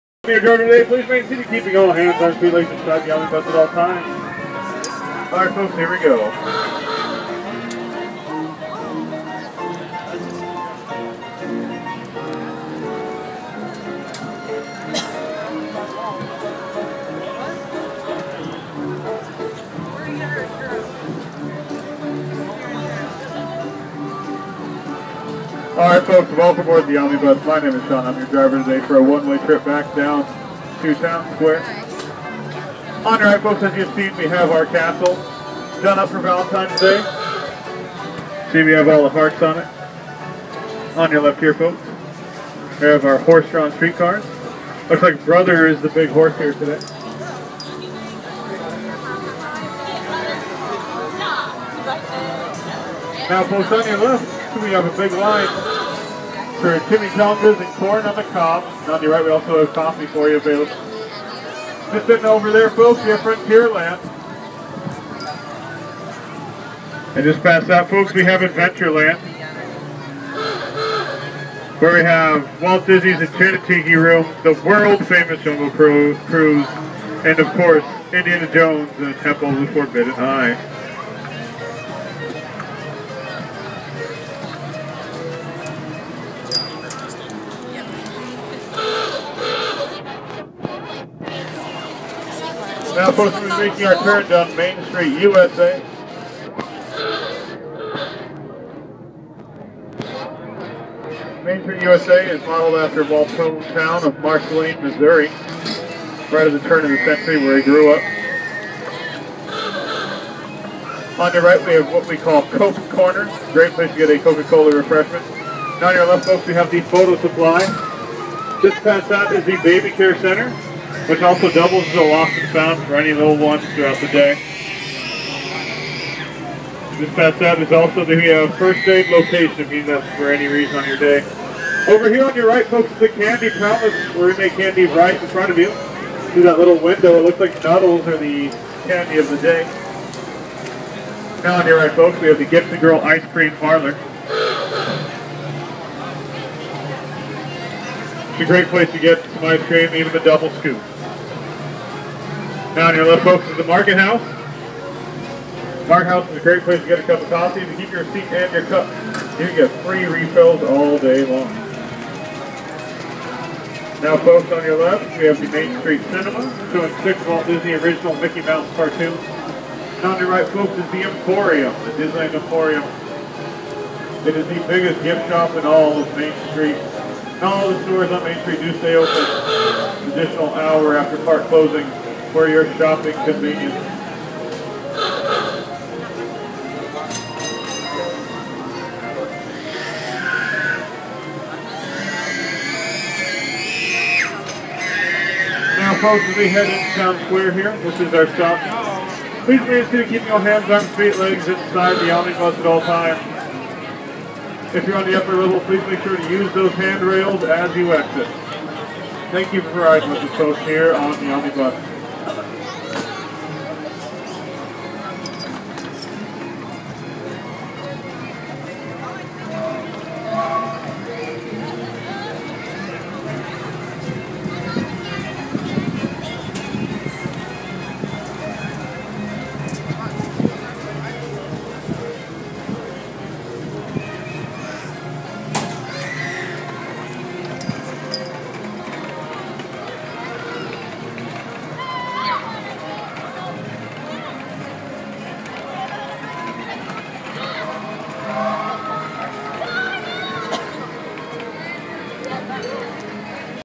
An Omnibus Ride Down Disneyland's Main Street USA
Take a ride down Disneyland's Main Street USA on the Omnibus, from the castle hub to the park entrance. Filmed from the vehicle's 2nd level in Feb 2013 by Seth Kubersky.